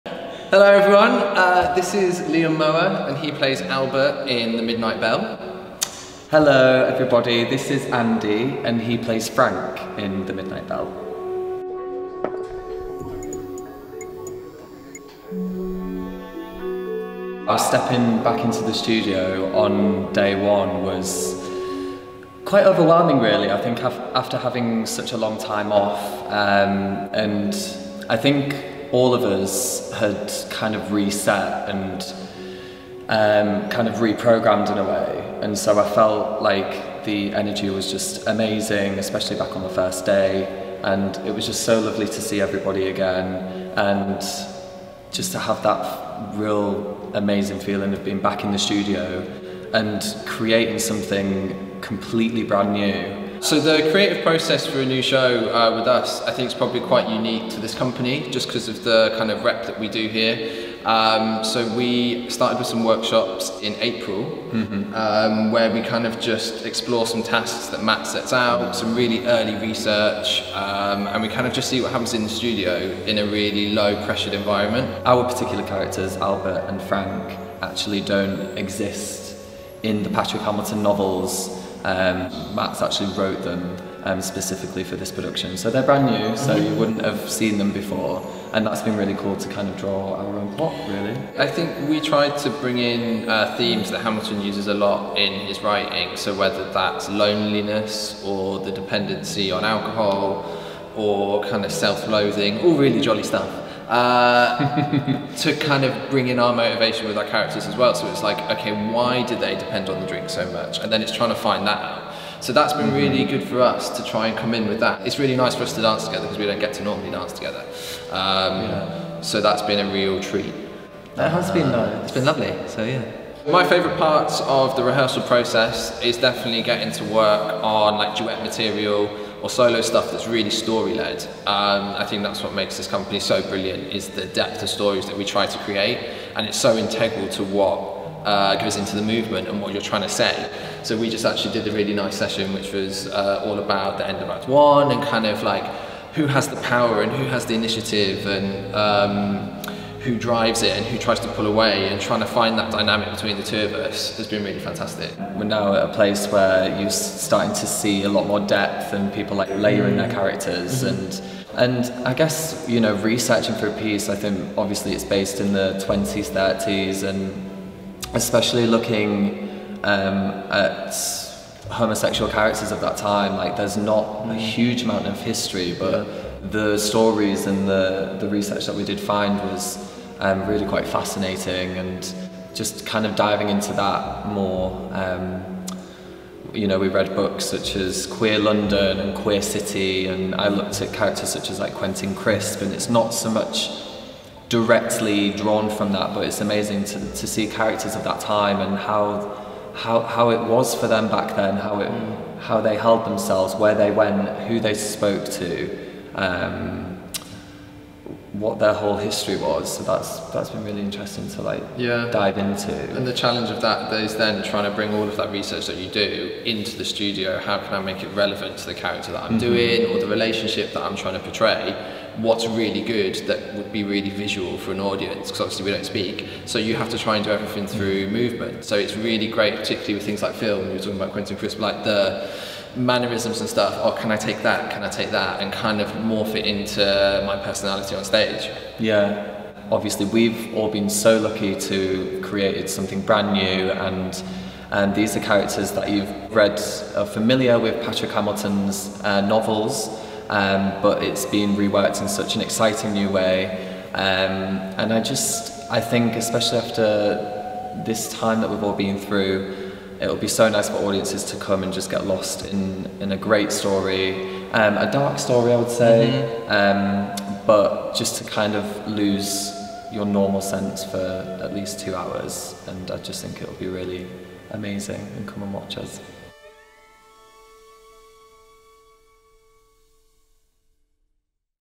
The Midnight Bell | Meet the Dancers: Andy Monaghan & Liam Mower
We sat down with Andy Monaghan and Liam Mower during rehearsals for The Midnight Bell to find out more about their characters "Frank" and "Albert". Tune in and find out what it's been like creating a new show with Matthew Bourne!